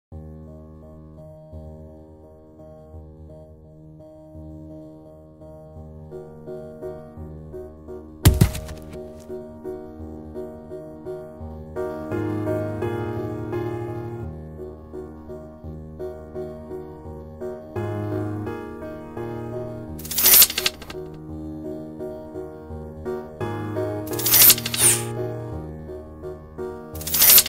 [stop motion] songoku vs batman
batman vs son goku qui gagneras? pour moi le choix est très vite fais  ^^ et pour vous?

bon visionnage